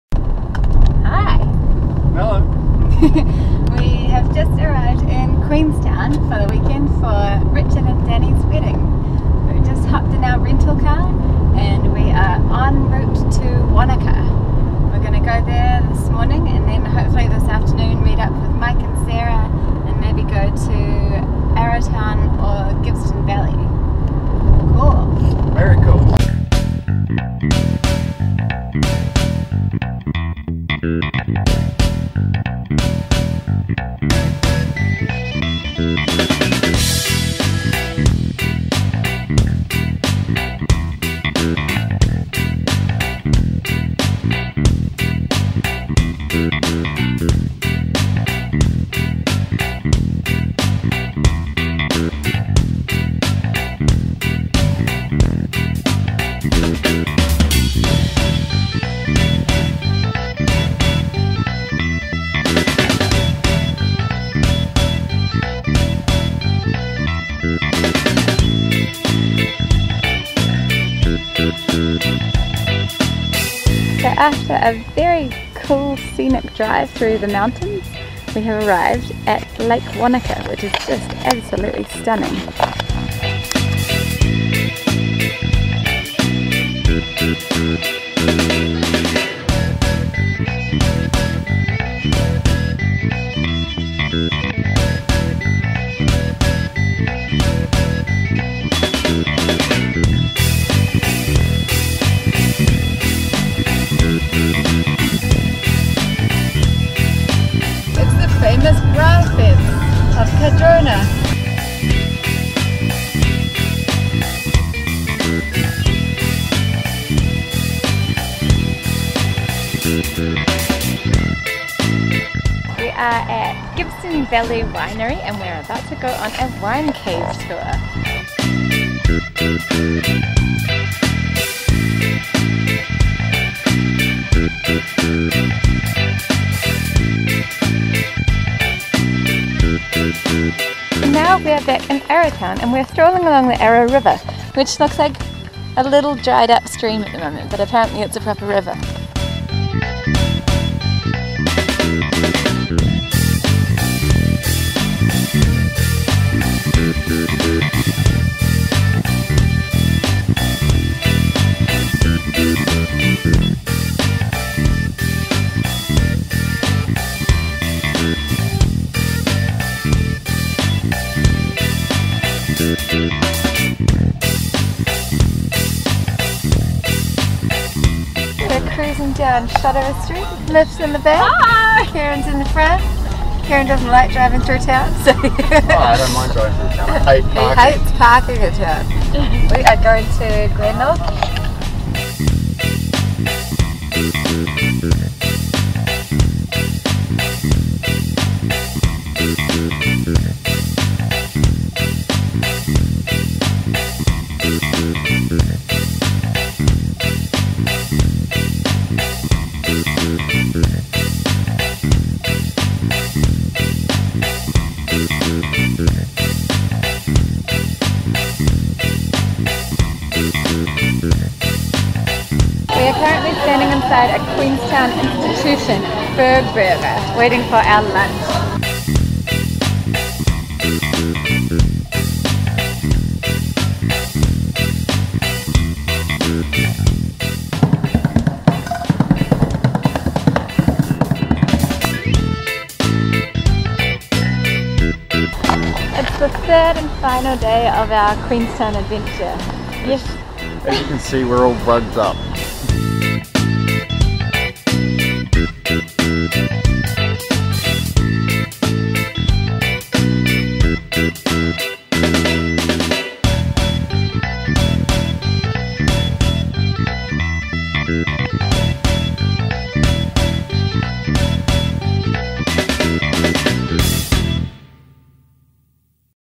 Queenstown 2014
Nix and I went to Queenstown for a friends wedding, we did some sightseeing whilst we were there. Arrowtown, Glenorchy, Wanaka to name a few. Queenstown is a stunning place and well worth visiting if you come to New Zealand.